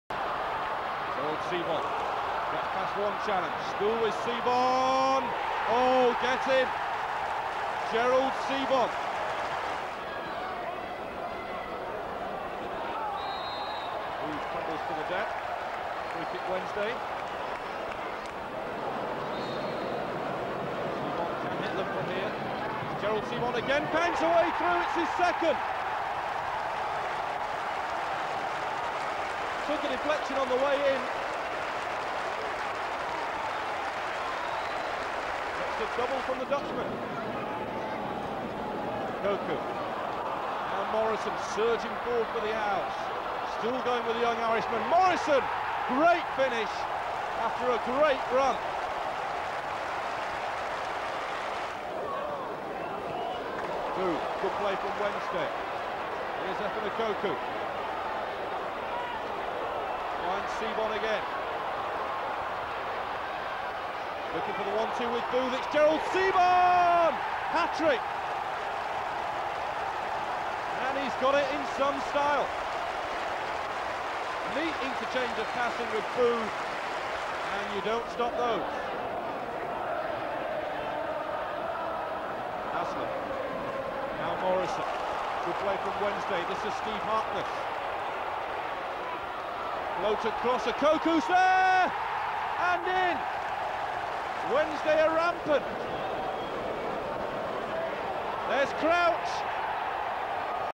WEDNESDAY 5-2 QUEENS PARK RANGERS, DIVISION 1, 2/12/2000
Gerald Sibon has one of his 'on' days, grabbing a hat-trick against the Londoners to move Wednesday up to 17th.